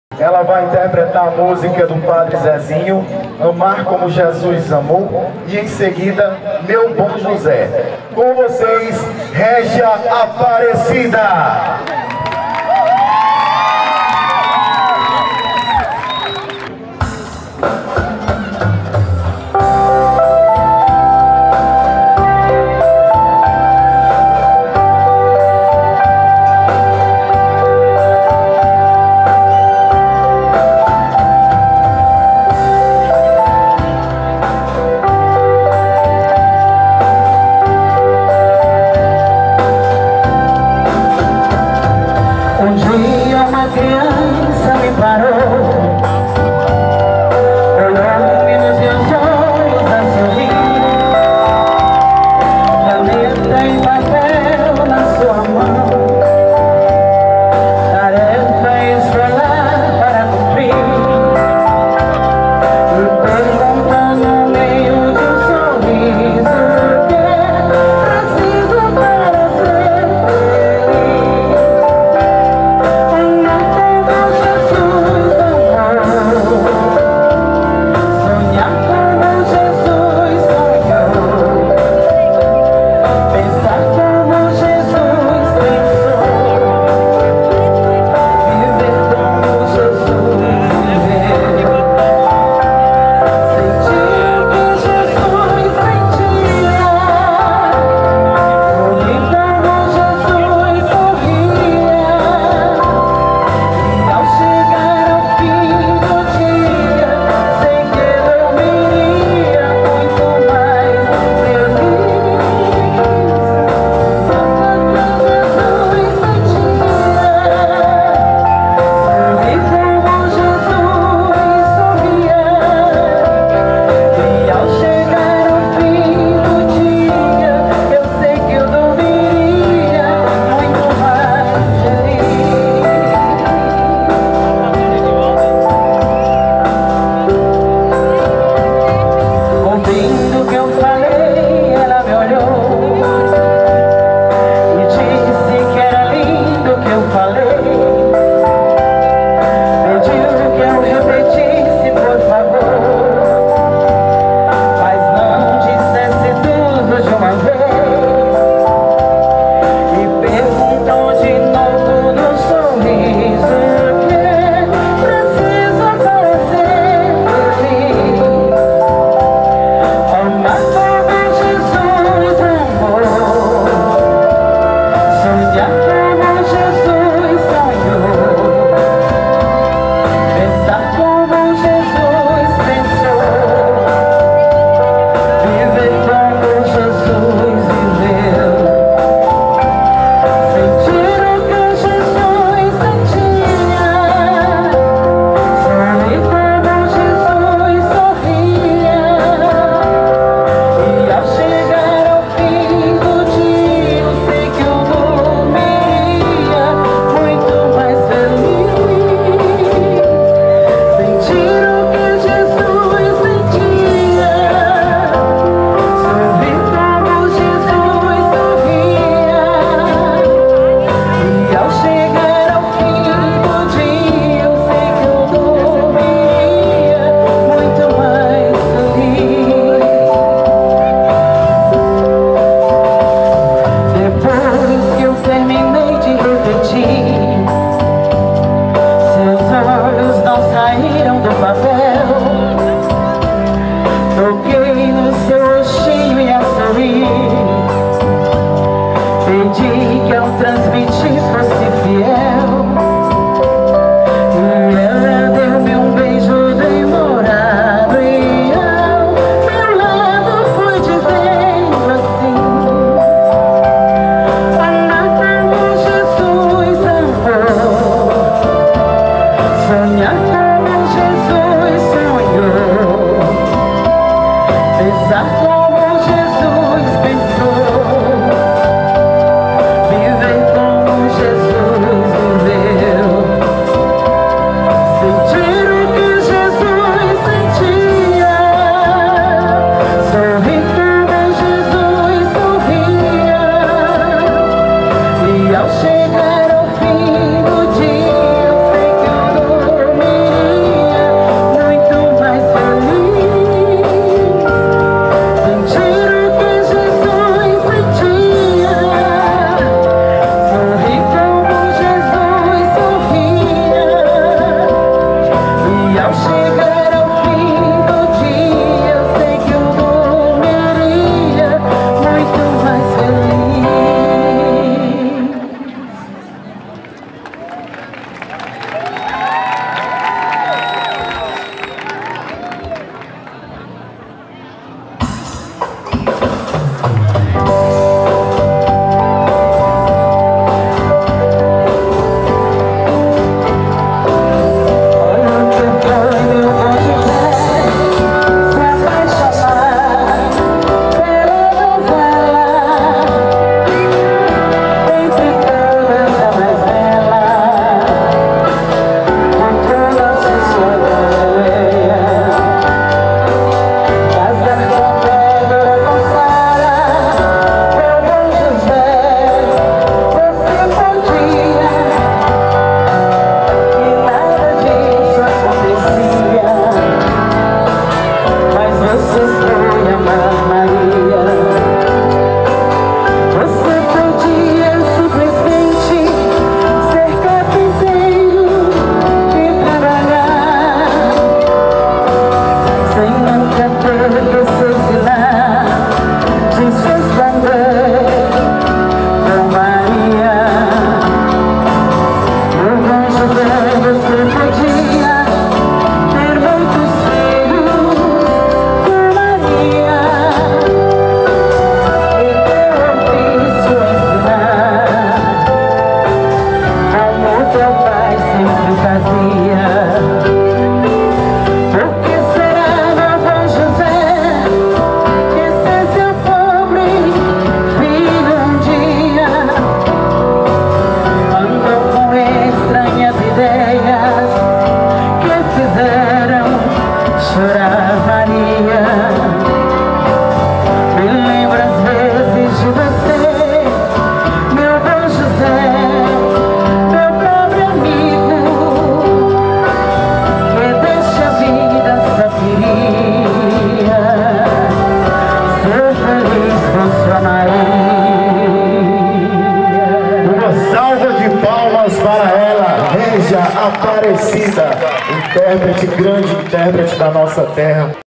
Show "Viva BOM JESUS 2017"
Régia Aparecida participou do Show Viva Bom Jesus, que é uma homenagem que a prefeitura realiza no dia 15 de dezembro e grandes artistas se apresentaram.